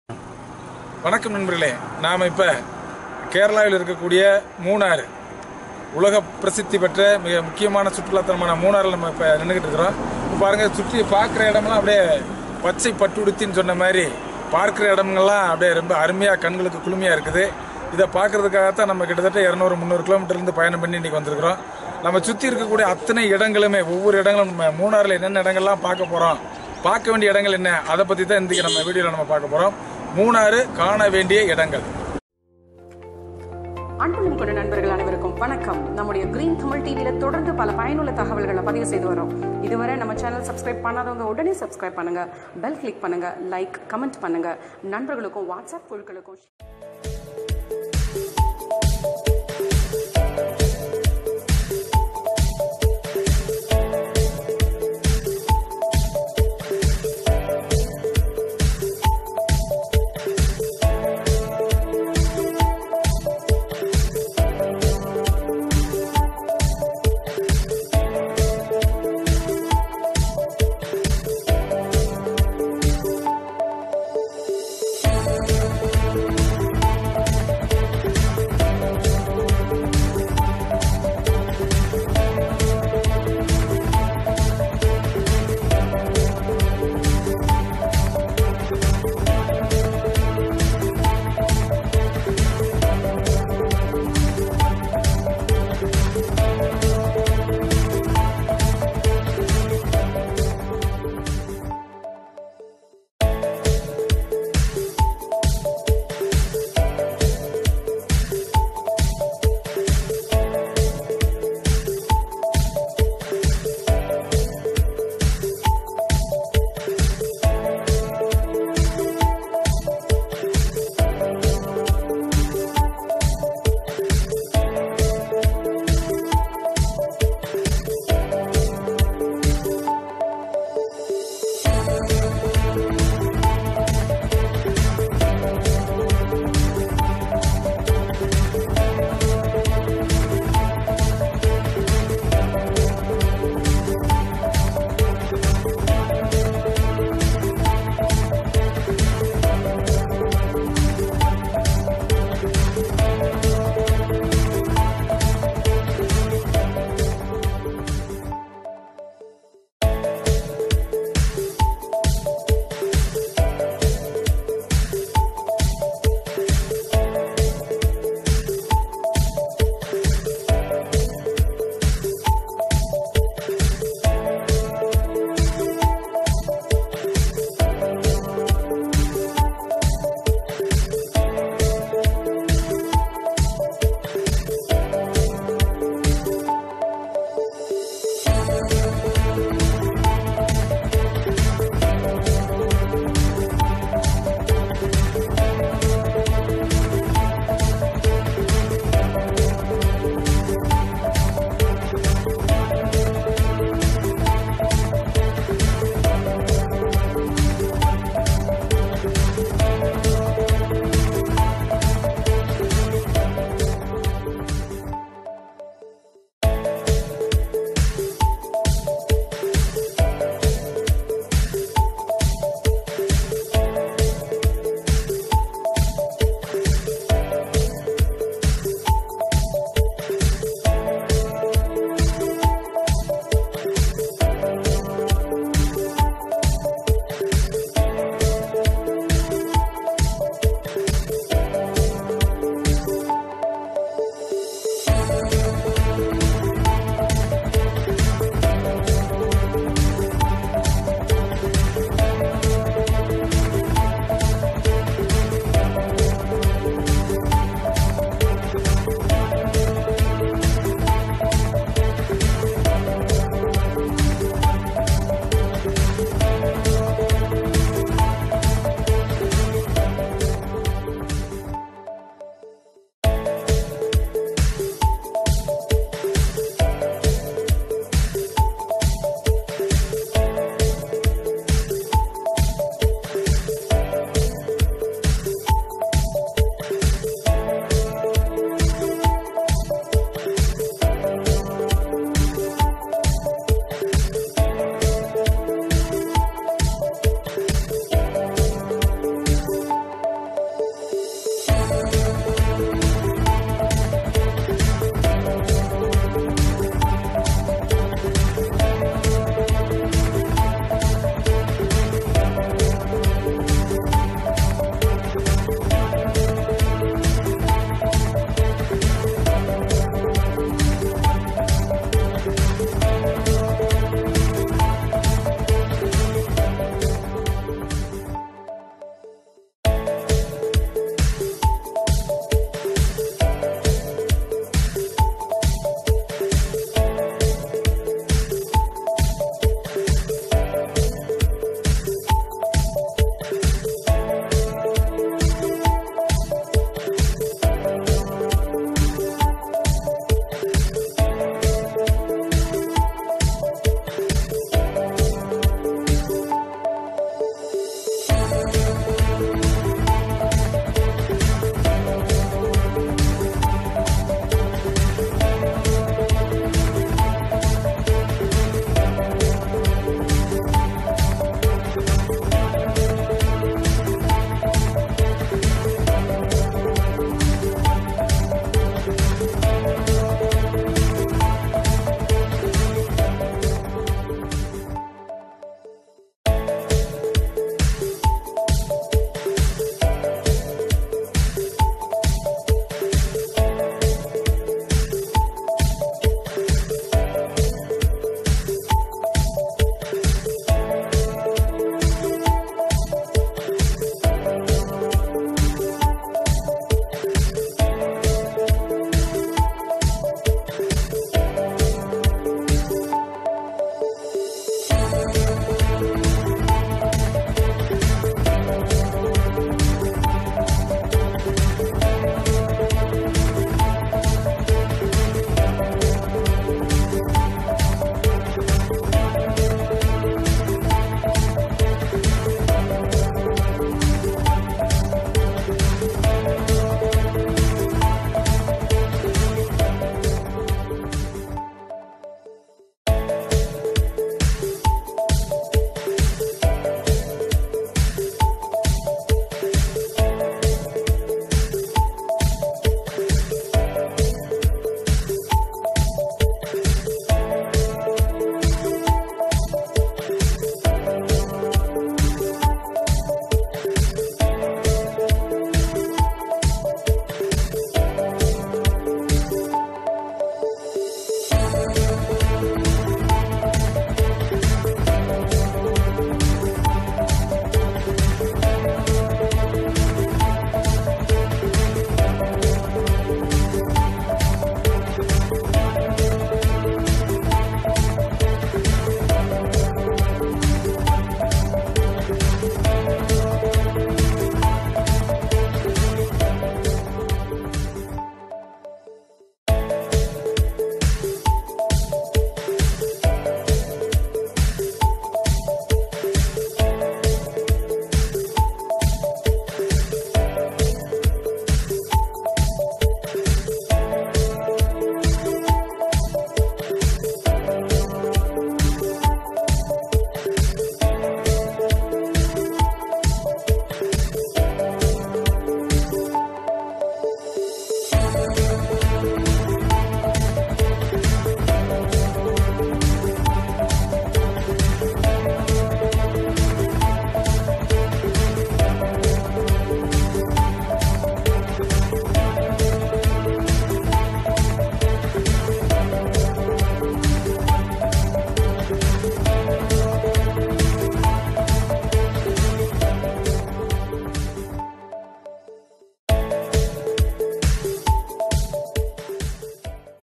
மூணார் சுற்றுலா - பார்க்க வேண்டிய இடங்கள் / Munnar Summer Tour - Kerala
மூணார்.